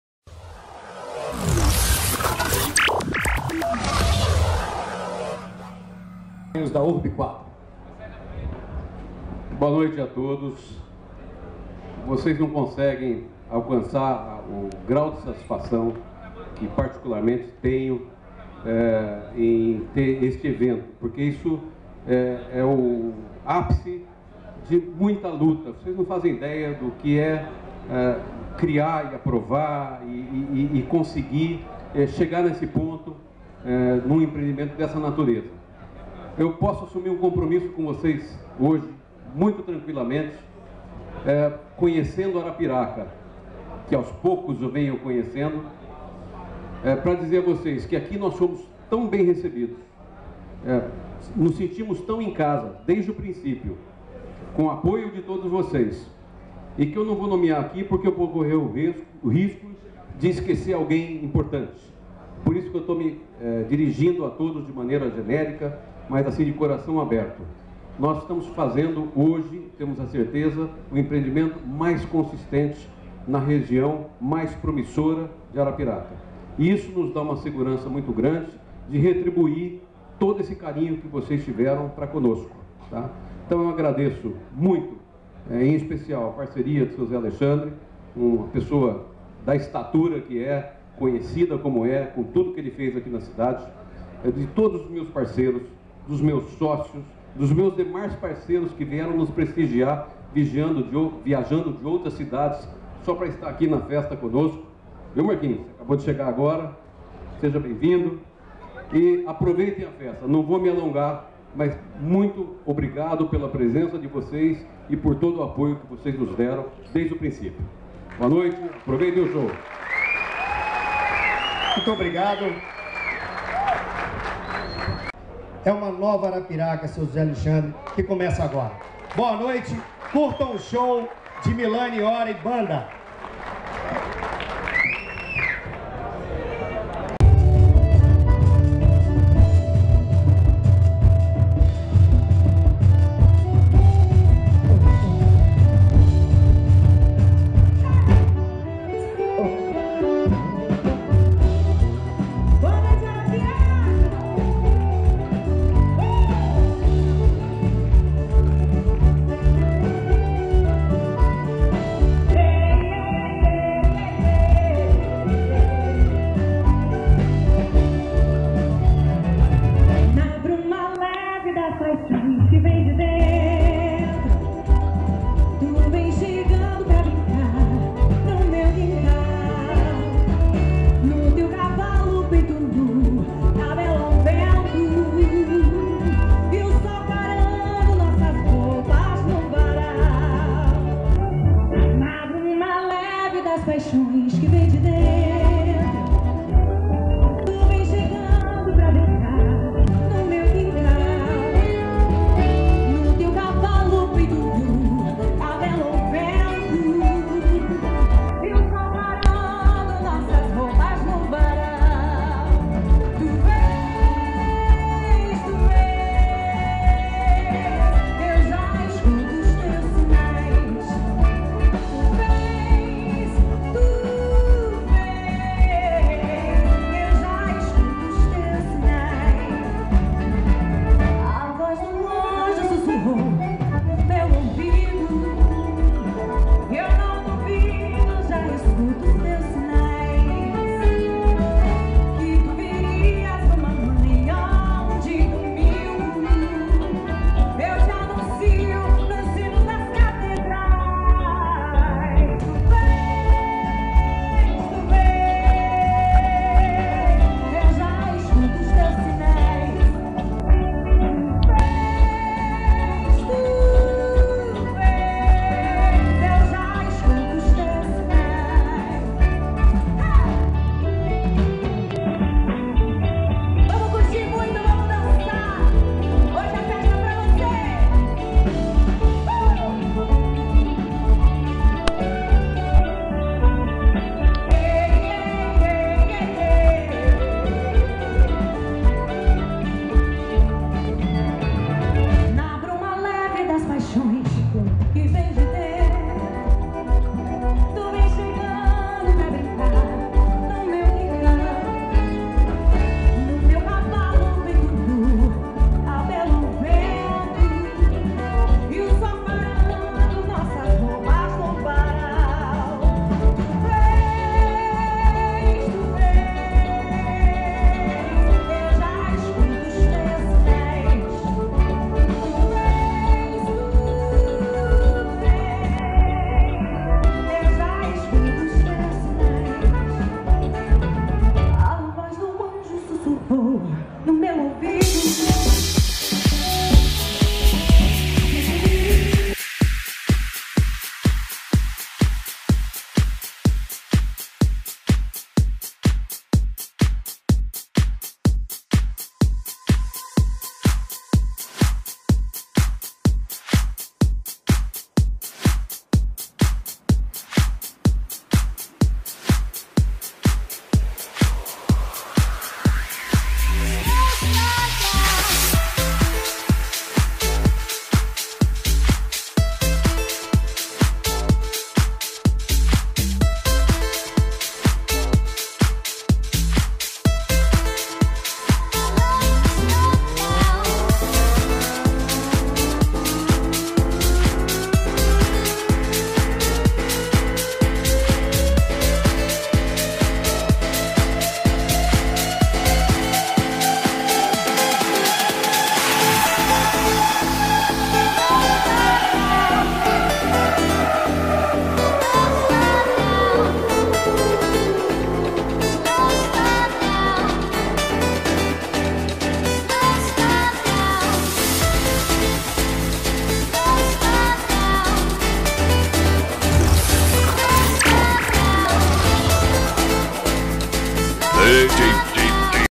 LANÇAMENTO DO RESIDENCIAL RIVIERA EM ARAPIRACA-AL
LANÇAMENTO DO RESIDENCIAL RIVIERA NO LAGO DA PERUCABA EM ARAPIRACA TEVE A PARTICIPAÇÃO ESPECIAL DA CANTORA MILLANE HORA E BANDA
IMAGEM DE JEAN NOTICIAS SEMPRE DE OLHO
19/03/2015